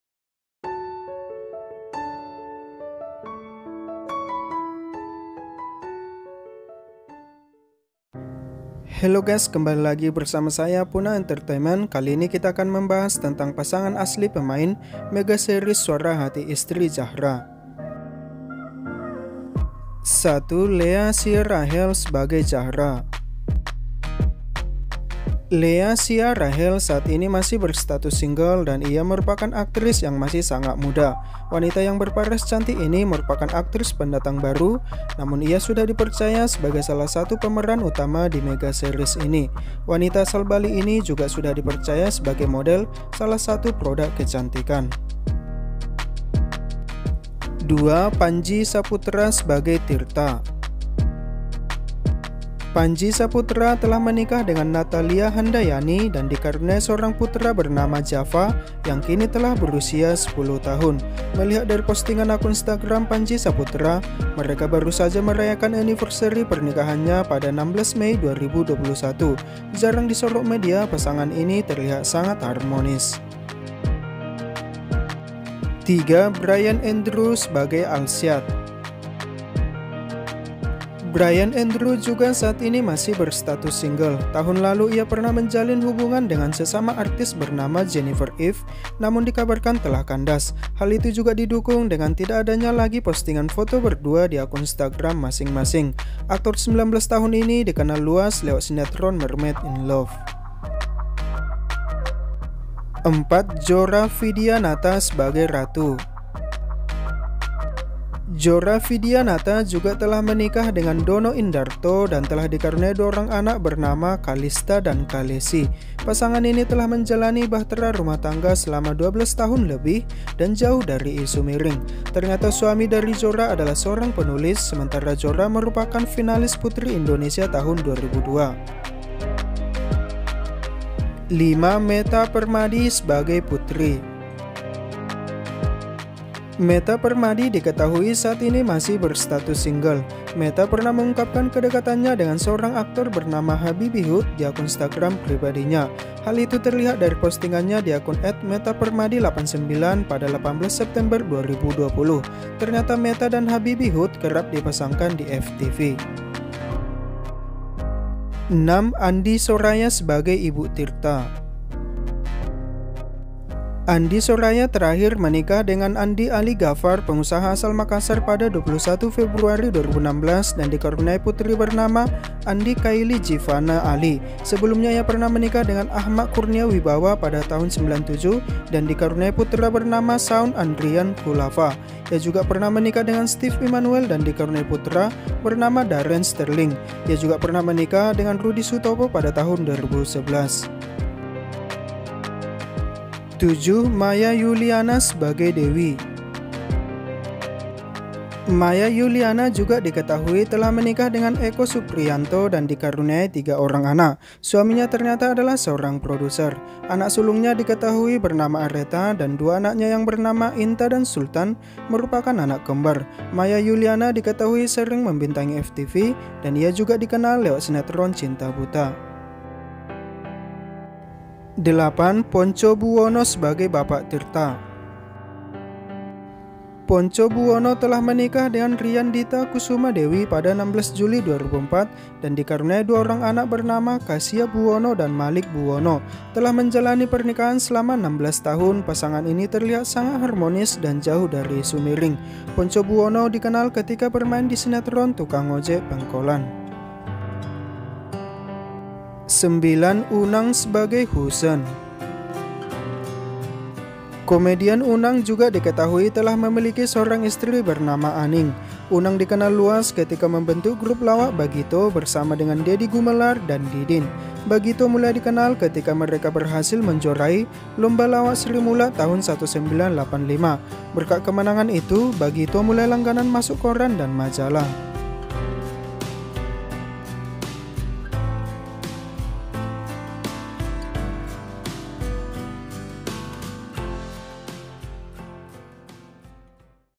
Pasangan Asli Pemain Mega Series SUARA HATI ISTRI ZAHRA INDOSIAR. Setiap Hari Pukul 18.00 WIB
Sinetron ini dibintangi oleh Lea Ciarachel, Panji Saputra, Bryan Andrew, Metta Permadi dan yang lainnya.